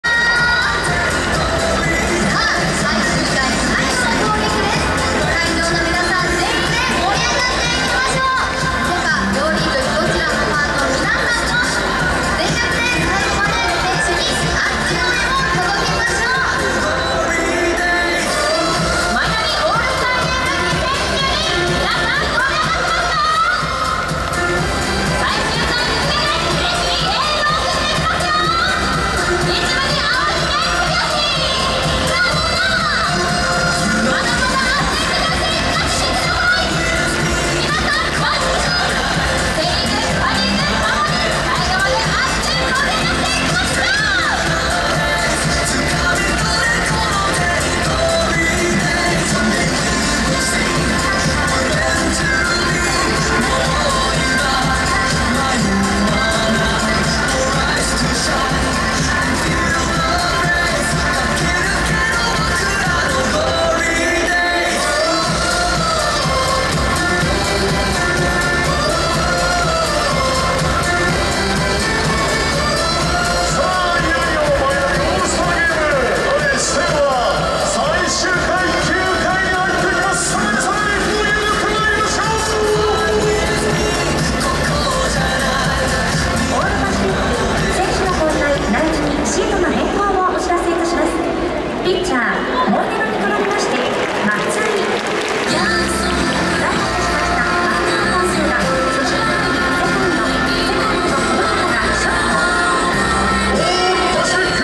2022.07.26 オールスター、最後のイニング間演出
ハリセンとかフラッグとかパ・リーグは球団の応援の特徴が出てますね。